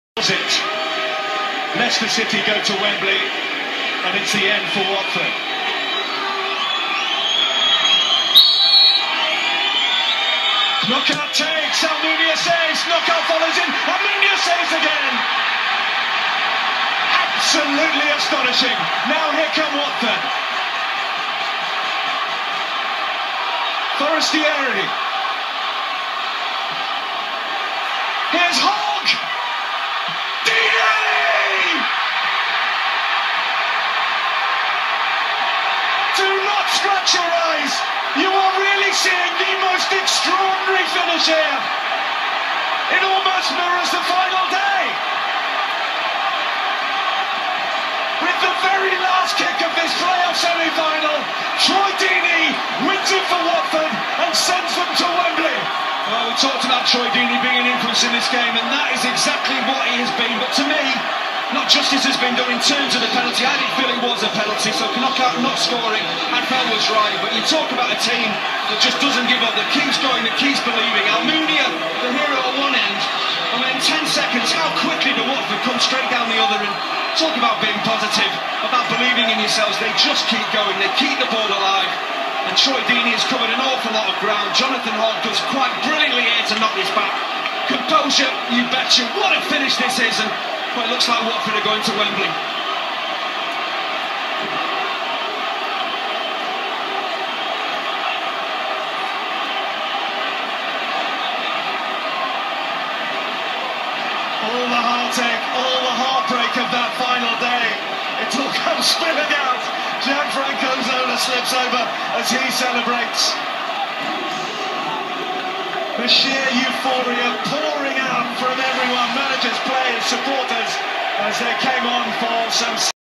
Watford vs Leicester City 13/05/2015
Troy Deeney scores Watford's winner in a stunning finish to their Championship play-off semi-final against Leicester